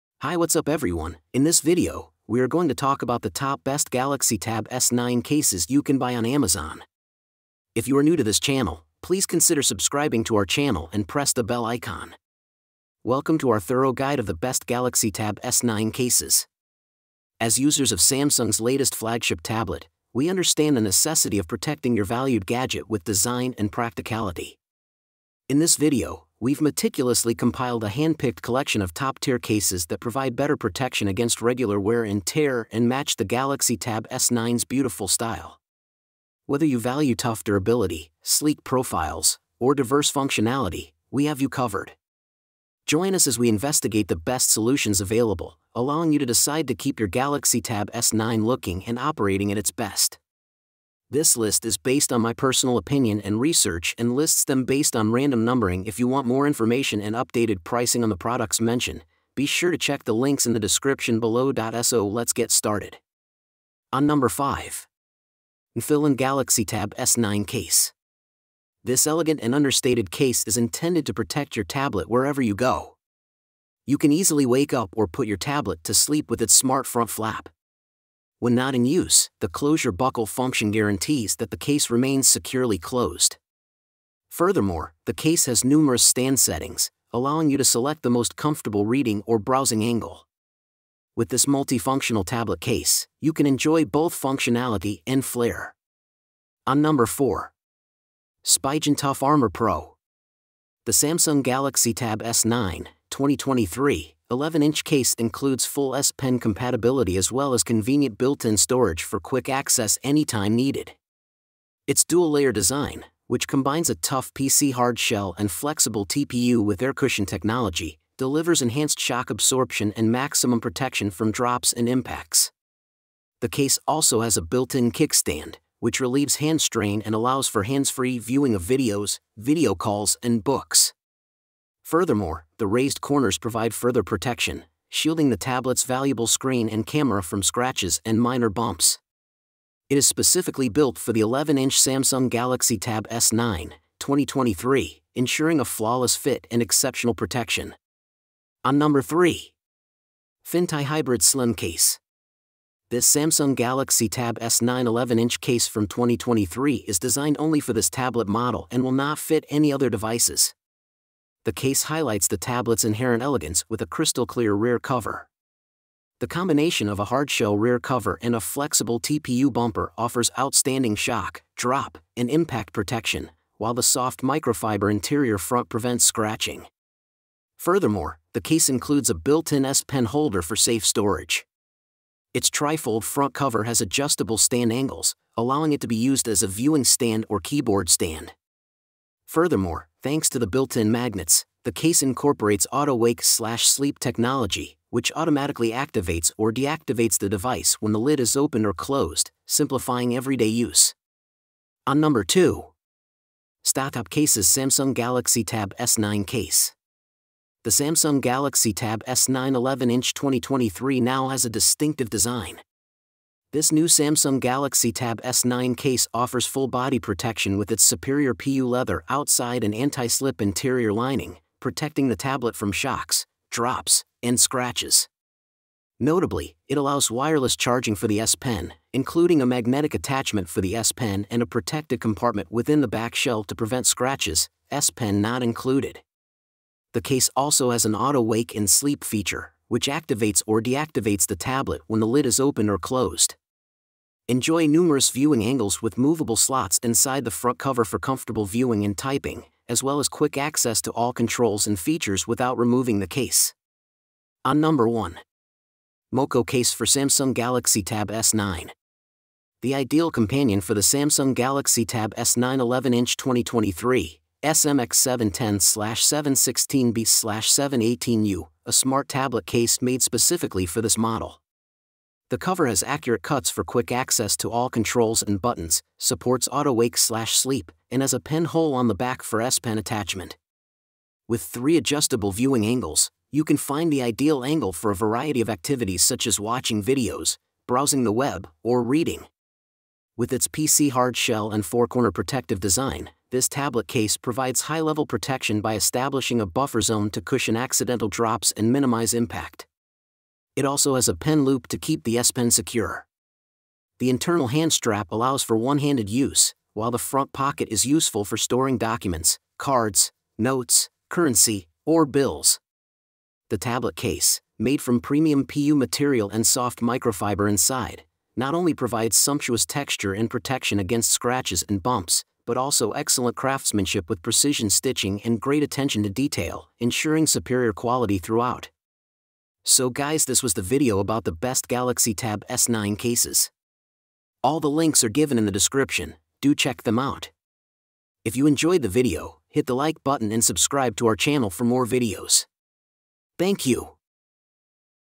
Top 5 Best Galaxy Tab S9 Cases [2025] 🔥 Protective | Spigen | Foldable
Let's talk about the Best Galaxy Tab S9 Cases. I have made a list of Best Galaxy Tab S9 Cases. which will help you decide which one to choose.

Quick Links: ✅✅✅

Introduction 0:00

5: NFILAND Galaxy Tab S9 Case 1:03
4: Spigen Tough Armor Pro 1:36
3: Fintie Hybrid Slim Case 2:28
2: ZtotopCases Samsung Galaxy Tab S9 Case 3:21
1: MoKo Case for Samsung Galaxy Tab S9 4:14

Welcome to our thorough guide of the best Galaxy Tab S9 cases! As users of Samsung's latest flagship tablet, we understand the necessity of protecting your valued gadget with design and practicality. In this video, we've meticulously compiled a handpicked collection of top-tier cases that provide better protection against regular wear and tear and match the Galaxy Tab S9's beautiful style. Whether you value tough durability, sleek profiles, or diverse functionality, we have you covered. Join us as we investigate the best solutions available, allowing you to decide to keep your Galaxy Tab S9 looking and operating at its best.

I have prepared this video guide to Best Galaxy Tab S9 Cases  to give you the ins and outs to help you make the right choice. So let's get started.